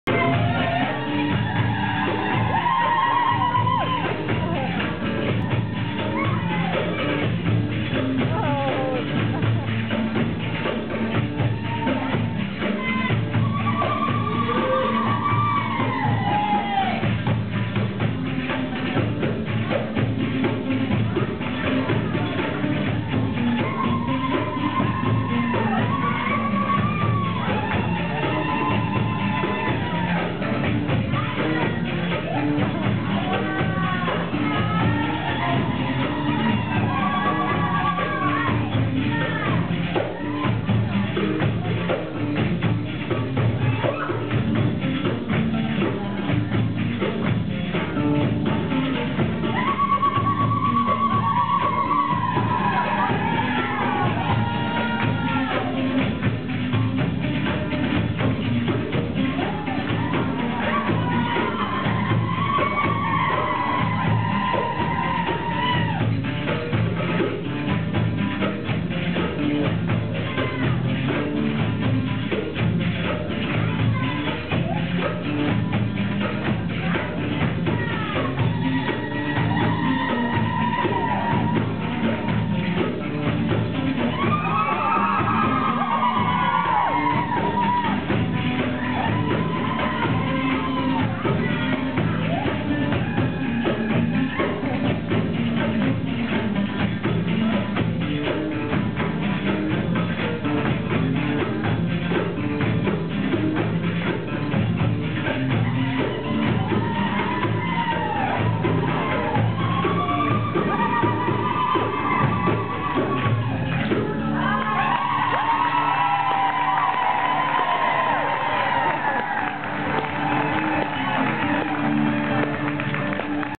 Arts on Fire 2010: Asia belly dance with turkish drop
Arts on Fire 2010: Asia belly dance with turkish drop. April 9, 2010 Pacifica, CA